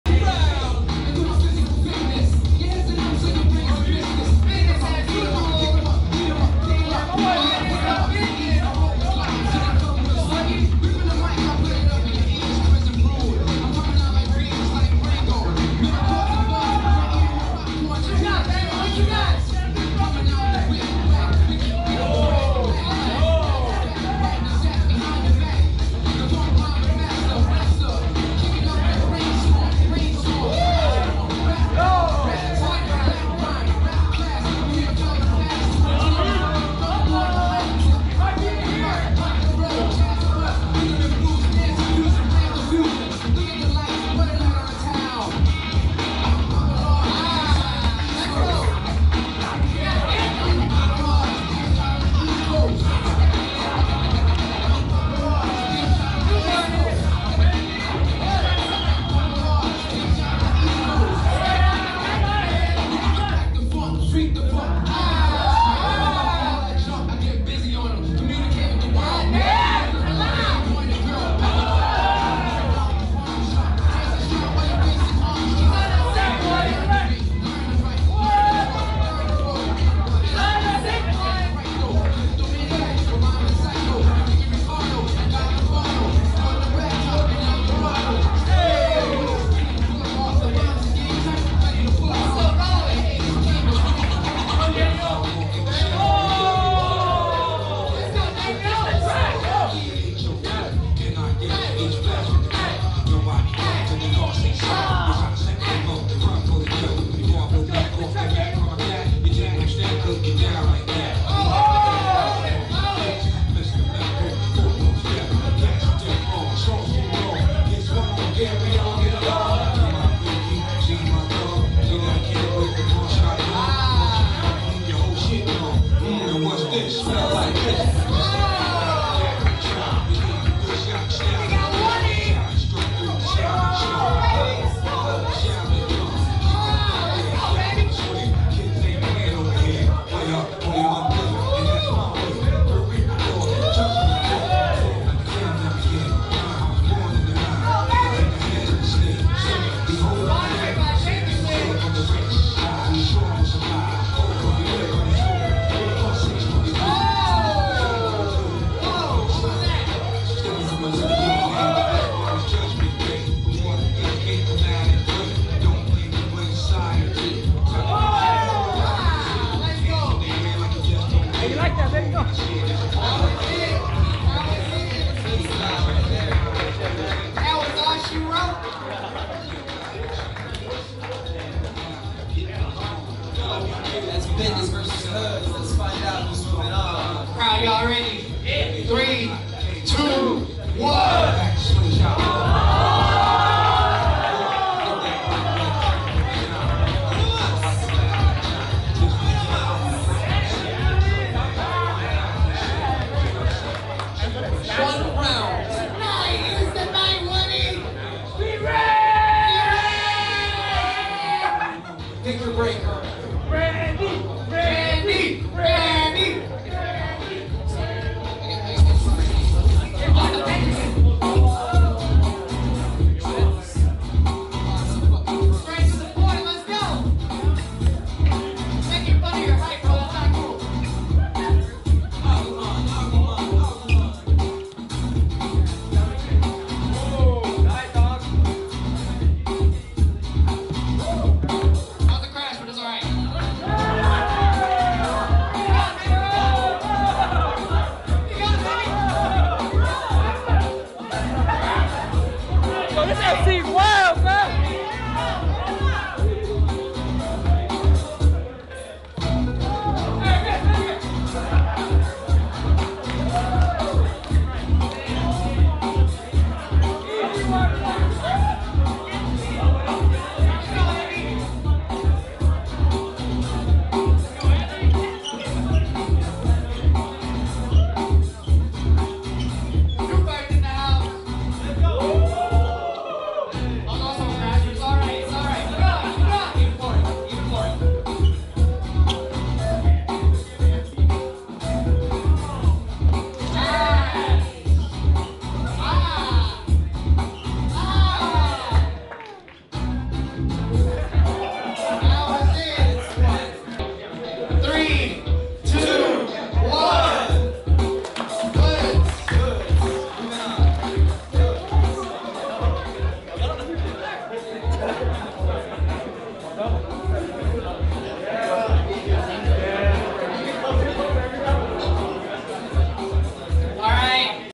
Hoodz Vs Binnez As Usual - 2 V 2 Top 8 - Heat Check 3
Heat Check Volume 3
2 V 2
Winner:
Hoodz

Judges:
Tafiya
Dyno
Run

DJ:
MagicSean
JayLo

MC:
Guppy
Jake